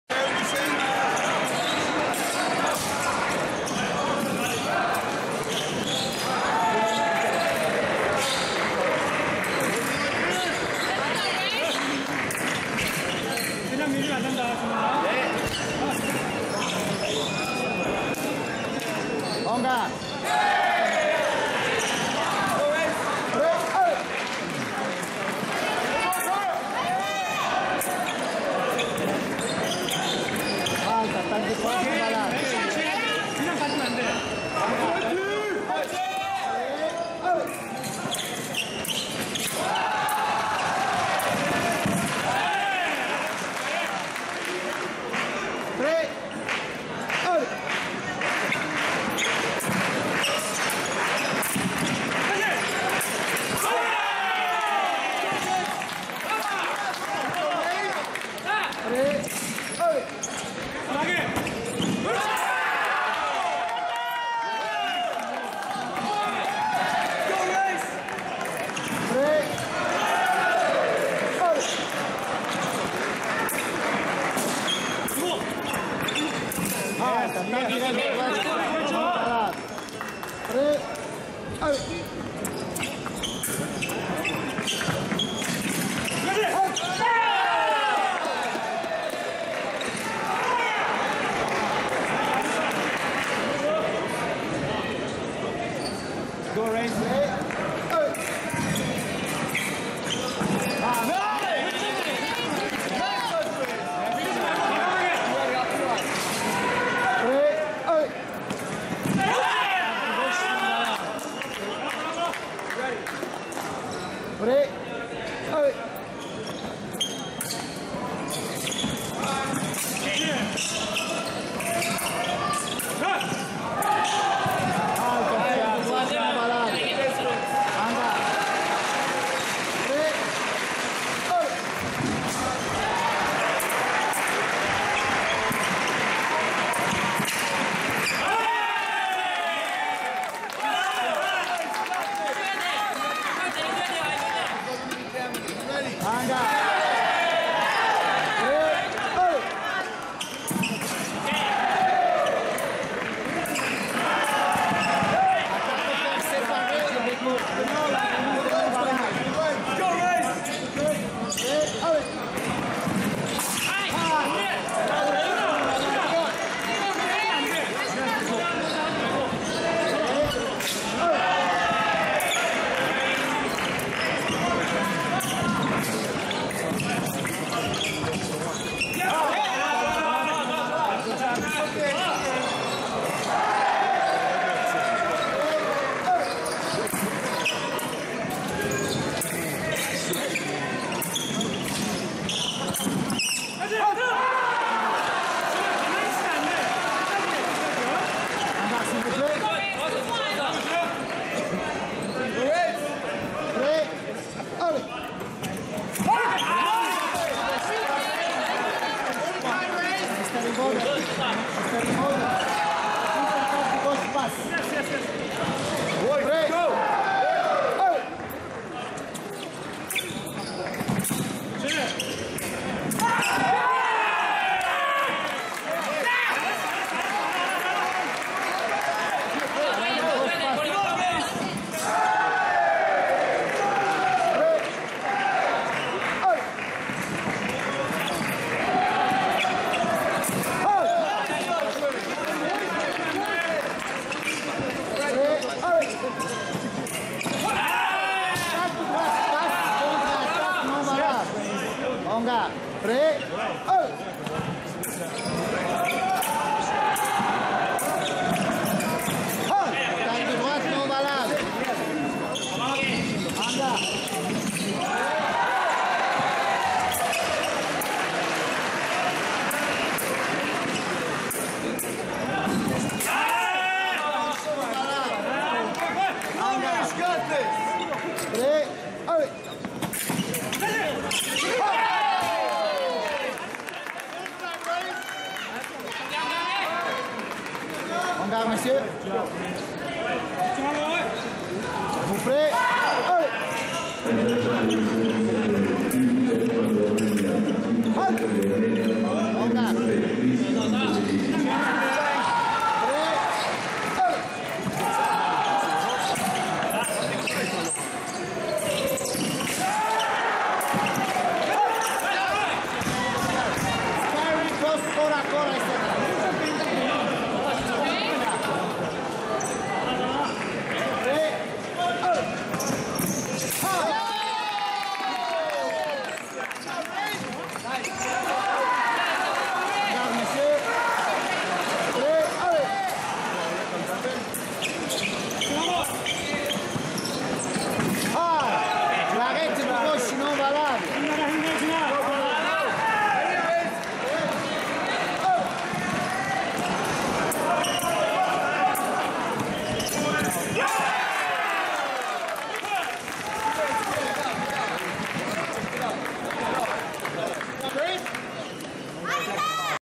2017, Jun Heo KOR vs Race Imboden USA
2017, World Championships, Leipzig, Quarter-Finals, Teams, KOR vs USA, Jun Heo KOR vs Race Imboden USA

Photo credit: Ferraro/Bizzi/FIE